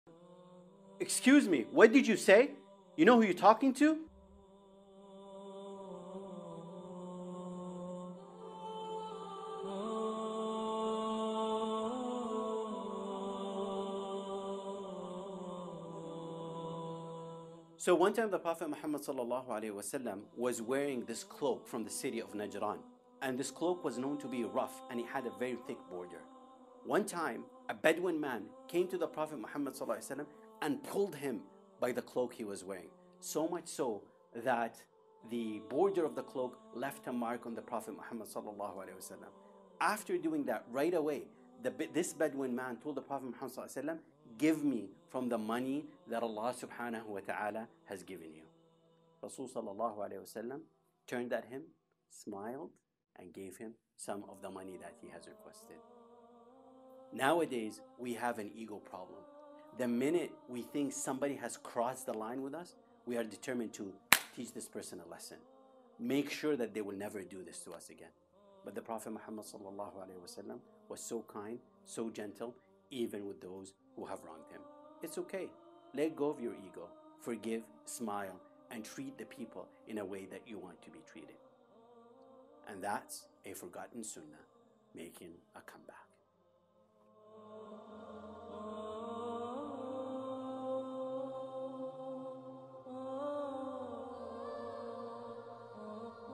Making a Comeback: The Forgotten Sunnah - Episode 4: When You're Wronged
Join us as we explore the Forgotten Sunnah of our beloved Prophet Muhammad (S.A.W.),  and how to implement them in this brand new series with our brother Hesham Alaghbari